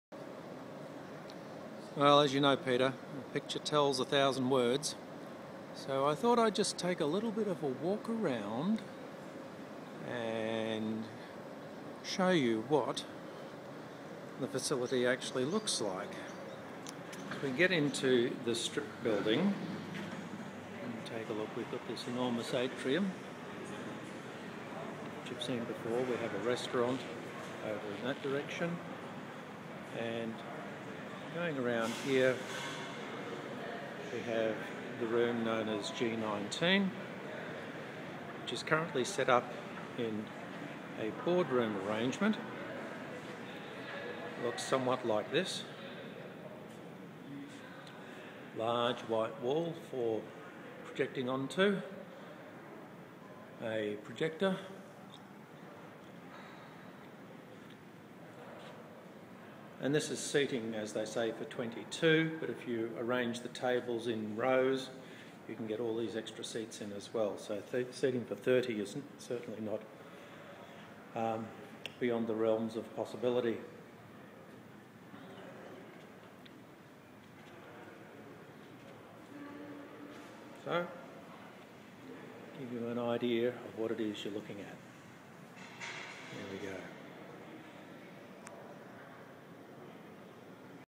Walkthru Monash Building 75 to Meeting room G19
Some people visiting Monash University for the DMFUG meeting on Feb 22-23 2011 will not have seen the building or room before. Here is a little walk thru.